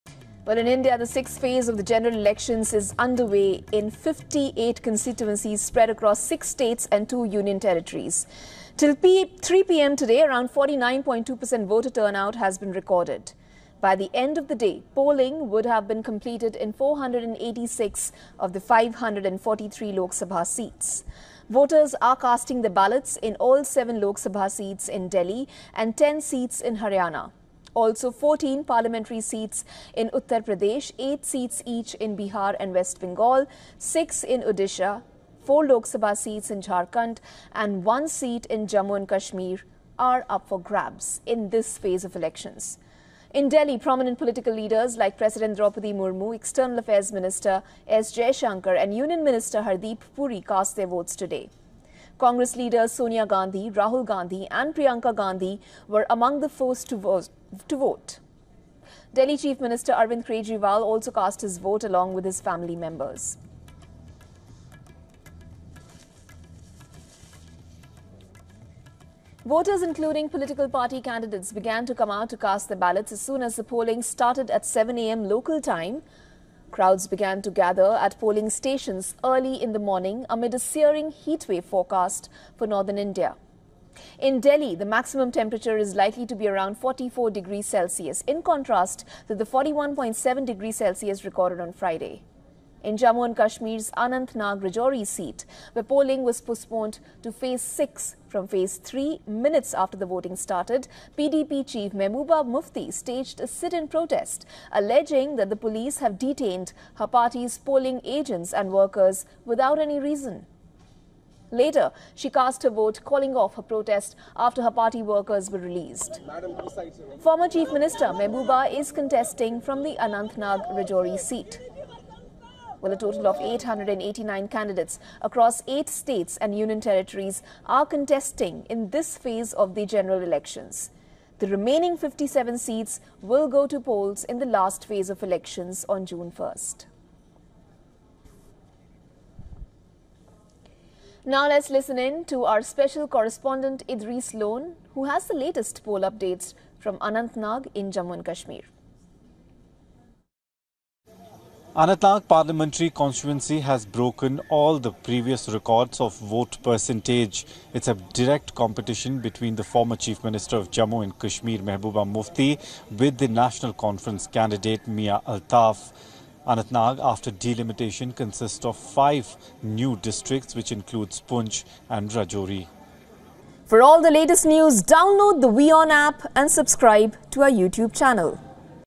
India General Election: Voting underway in 58 Lok Sabha seats, prominent leaders cast their votes
The voting for the sixth phase of Lok Sabha elections, which includes 58 seats across six states and two Union Territories - Delhi and Jammu and Kashmir, is currently underway. Watch in the report for more updates!